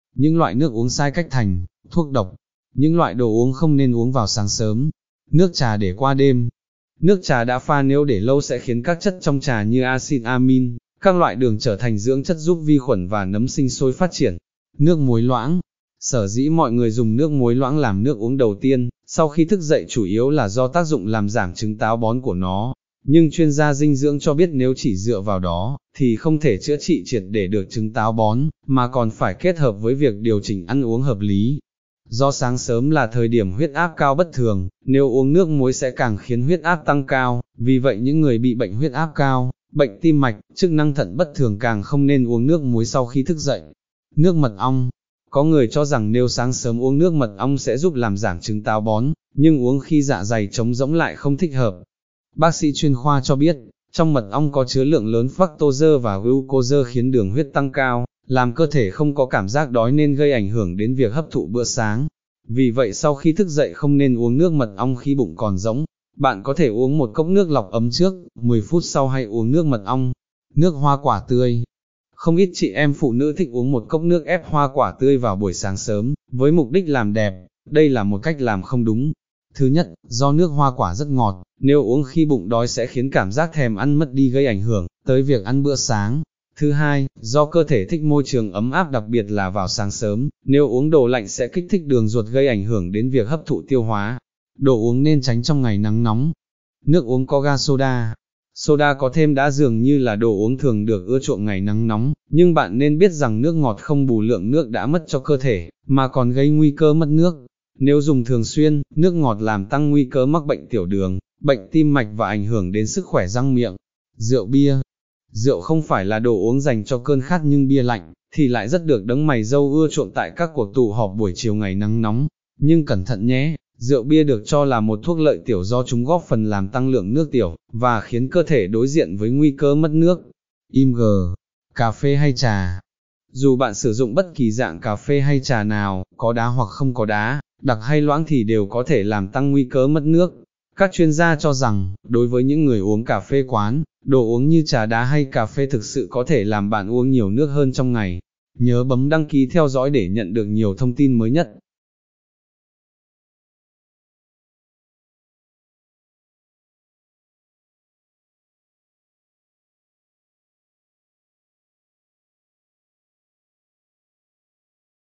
Những loại nước uống sai cách thành... thuốc độc
Có những loại nước uống tác dụng tốt cho sức khoẻ, nhưng nếu uống sai cách, sai thời điểm lại có thể trở thành 'thuốc độc', gây hại vô cùng cho cơ thể.